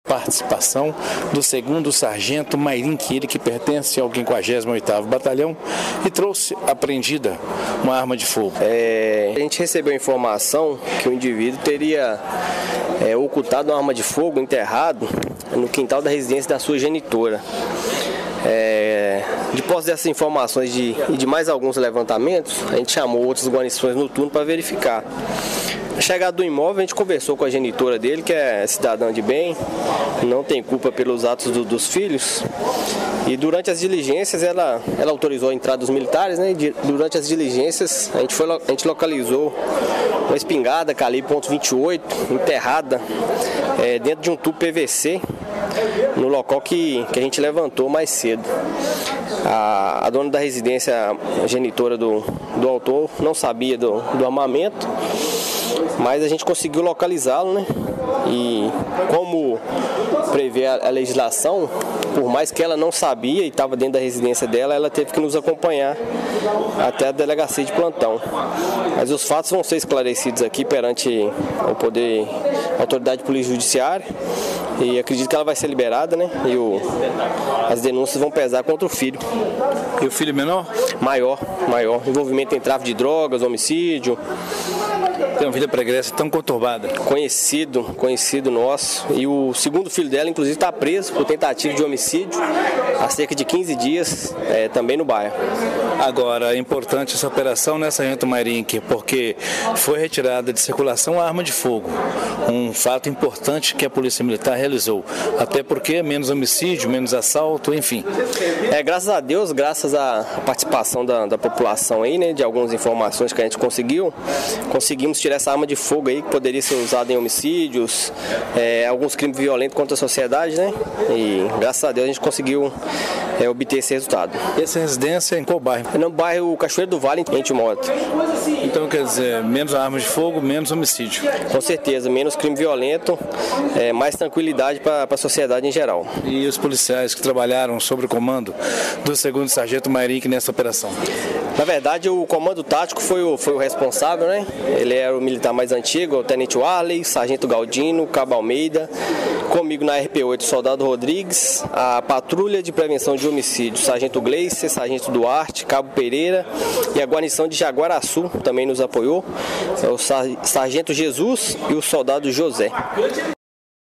Através de denúncias anônimas PM localiza arma de fogo em Timóteo.
TIMÓTEO - Por volta das 18h de ontem (26/06), populares fizeram denúncias que um rapaz de 24 anos, velho conhecido da polícia, havia escondido uma arma de fogo no quintal da residência da mãe dele situada na  Avenida Minas Gerais no distrito de Cachoeira do Vale em Timóteo.

Os militares foram conferir e informaram acerca da denúncia, a mulher de 43 anos mãe do denunciado, franqueou a entrada dos PMs para as buscas, no quintal da residência eles localizaram um tubo de pvc enterrado em local de difícil acesso, e dentro do tubo uma arma de fogo tipo espingarda com 02 cartuchos.

O suspeito denunciado não foi localizado, a mulher foi conduzida até à Delegacia juntamente com o material apreendido.